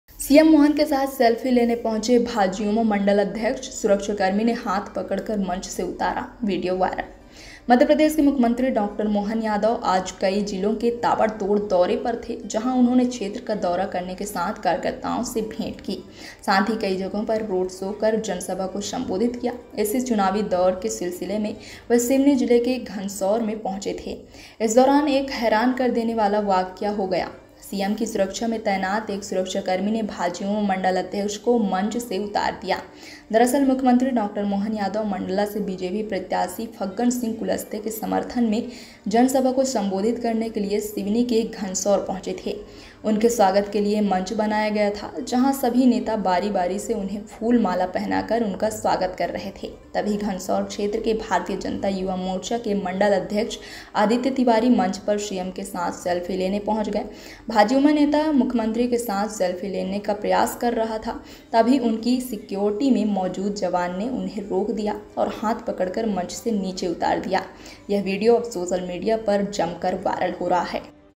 CM Mohan yadav के साथ सेल्फी लेने पहुंचा BJP मंडल अध्यक्ष, सुरक्षाकर्मी ने हाथ पकड़कर मंच से उतारा!
सिवनी। मध्य प्रदेश के मुख्यमंत्री डॉ मोहन यादव आज कई जिलों के ताबड़तोड़ दौरे पर थे जहां उन्होंने क्षेत्र का दौरा करने के साथ कार्यकर्ताओं से भेंट की। साथ ही कई जगहों पर रोड शो कर जनसभा को संबोधित किया। इसी चुनावी दौरे के सिलसिले में वह सिवनी जिले के घंसौर में पहुंचे थे
Suture. Madhya Pradesh Chief Minister Dr. Mohan Yadav was on a whirlwind tour of several districts today where he visited the areas and met the workers. He also addressed public meetings by holding road shows at many places. In connection with this election tour, he had reached Ghansaur in Seoni district.
सुझाव